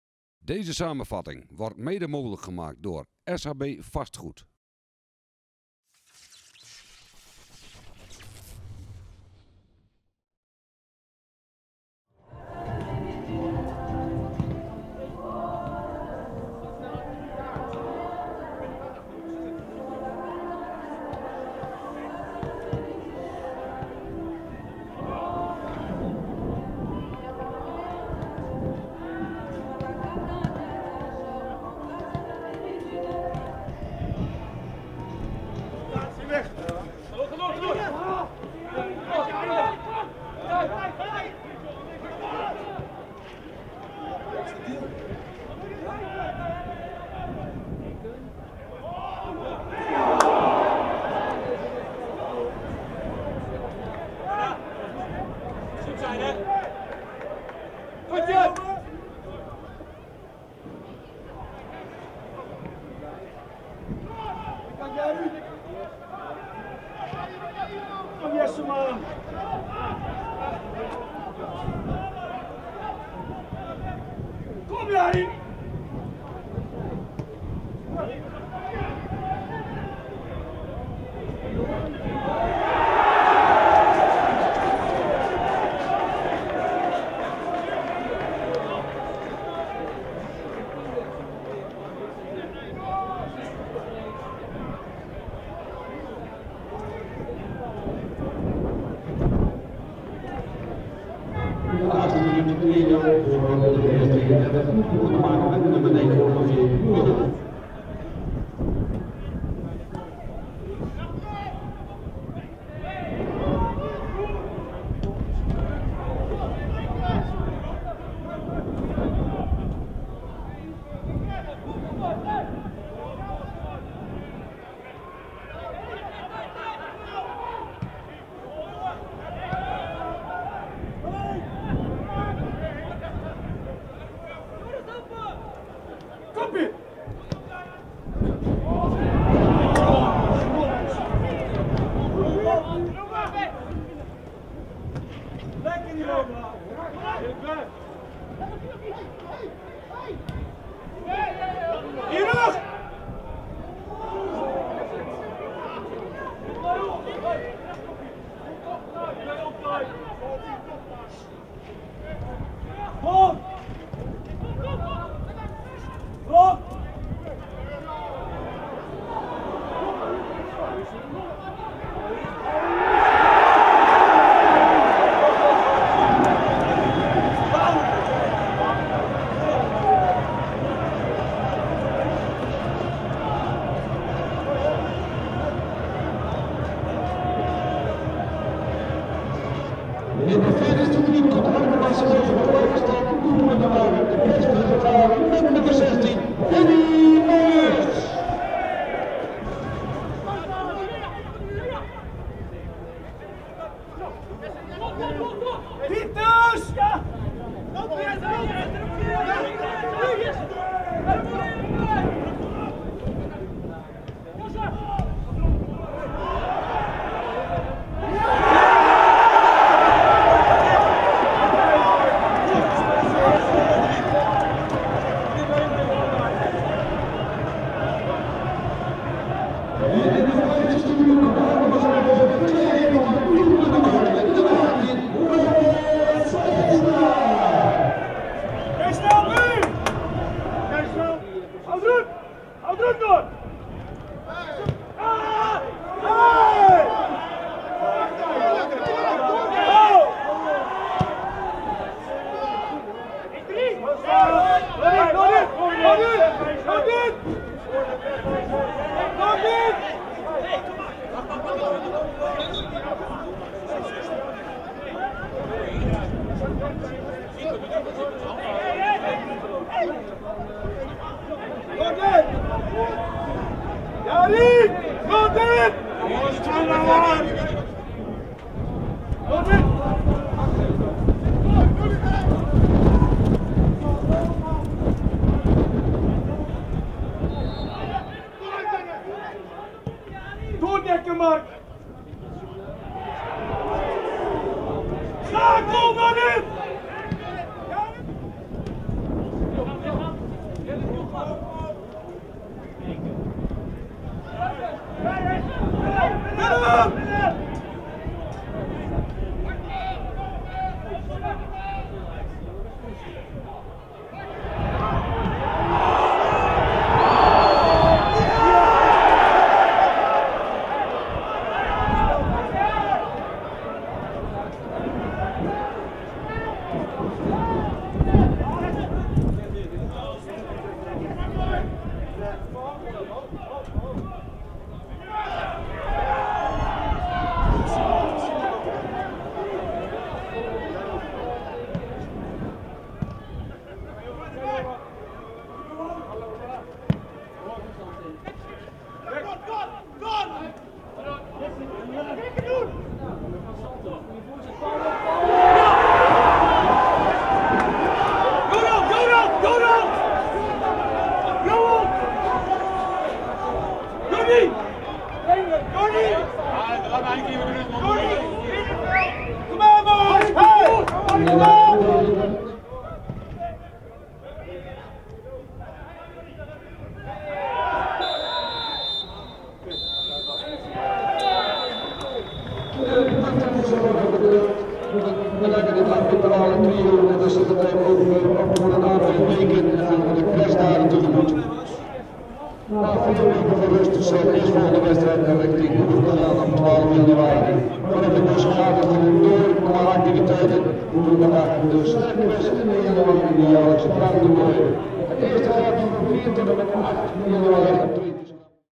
Harkemase Boys - DVS'33 ('18/'19)
In de laatste wedstrijd voor de winterstop speelde Harkemase Boys thuis tegen de ambitieuze titelkandidaat DVS'33 uit Ermelo.